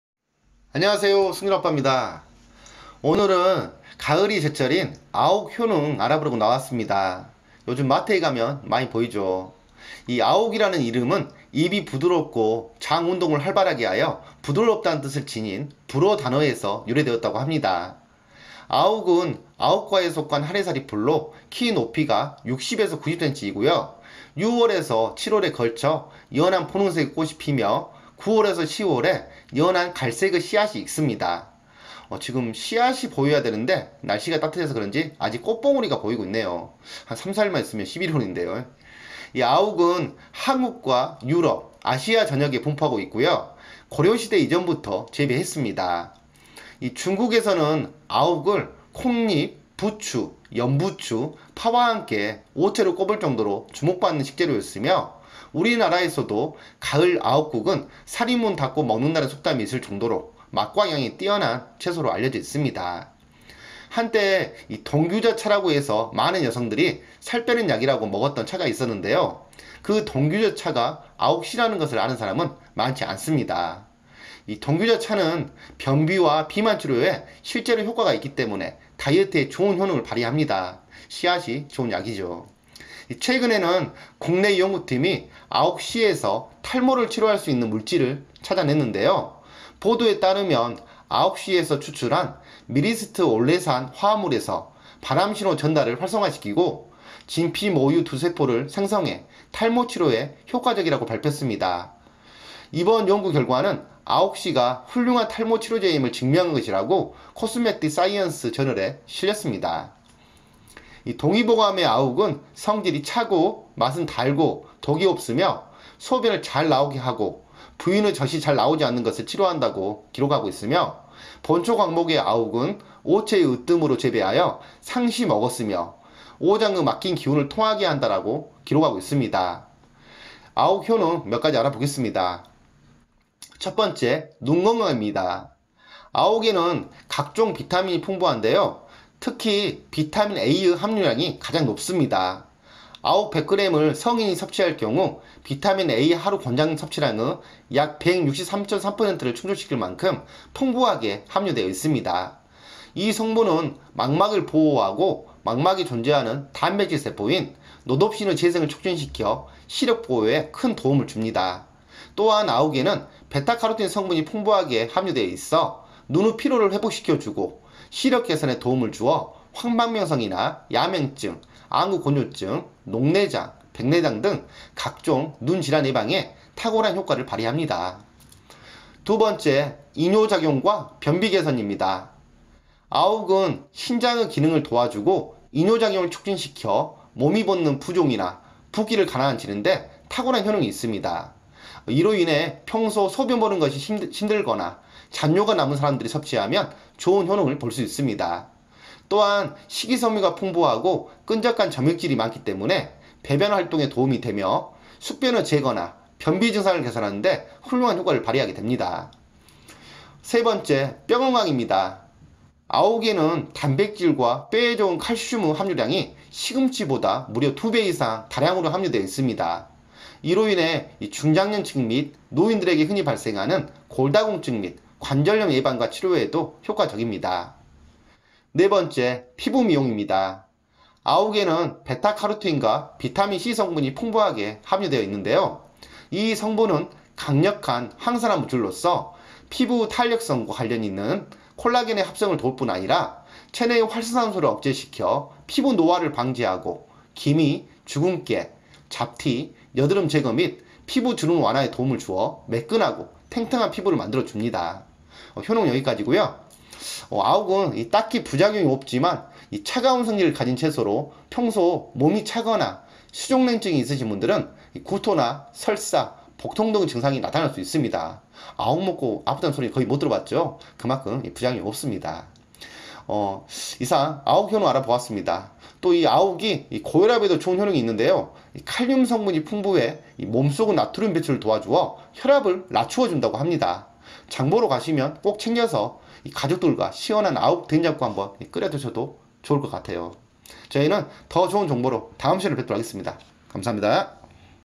60 70대의 피부를 20대로 만드는 이~씨앗!! 장복하면 몸이 가벼워지고 뼈가 무쇠처럼!!탈모에도 최고!!
한 때 '동규자(冬葵子)차'라고 해서 많은 여성들이 살빼는 약이라고 먹었던 차가 있었는데요. 그 동규자가 아욱씨라는 것을 아는 사람은 많지 않습니다.
동규자차는 변비와 비만치료에 실제로 효과가 있기 때문에 다이어트에 좋은 효능을 발휘합니다.
최근에는 아욱씨가 탈모를 치료할 수 있다는 연구결과~!!